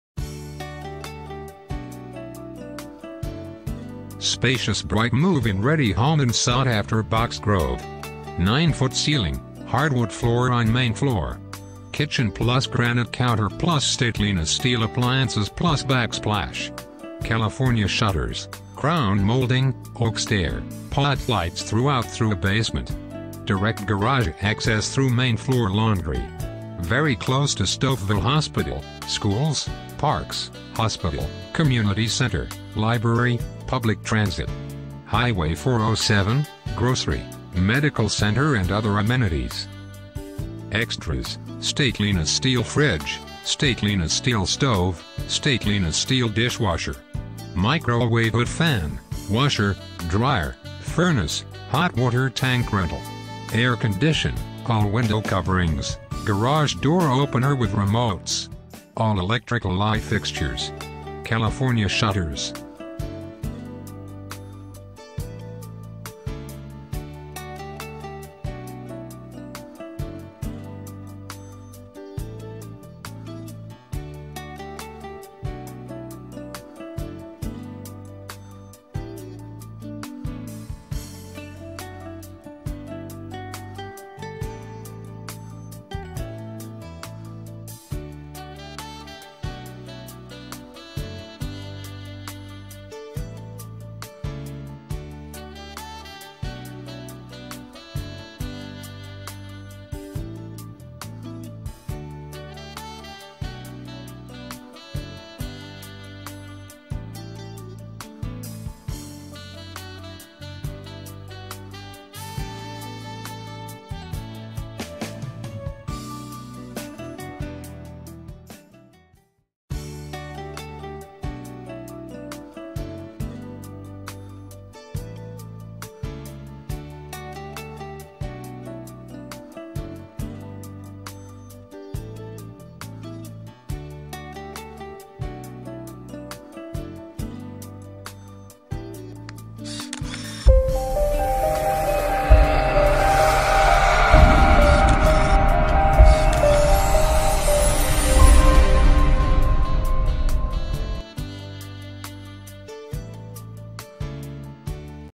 150 Oakborough Drive Markham-By-sjvirtualtours.ca
Spacious Bright Move-In Ready Home In Sought After Box Grove *9Ft Ceiling,Hardwood Floor On Main Floor, Kitchen +Granite Counter+ S/S Appliances+Backsplach, California Shutters, Crown Moulding, Oak Stair,Pot Lights Thru-Out Through Basement, Direct Garage Access Through Main Floor Laundry,Very Close To Stouffville Hospital,Schools, Parks, Hospital, Community Ctr, Library, Public Transit, Hwy 407, Grocery, Medical Blg & Other Amenities.

Extras:S/S Fridge, S/S Stove,S/S Dishwasher,Microwave Hood Fan, Washer, Dryer, Furnace,Hwt(R),Cac,All Window Coverings,Gdo With Remotes. All Electrical Light Fixtures. California Shutters, Seller & Agent Do Not Warrant Retrofit Status Of Bsmt.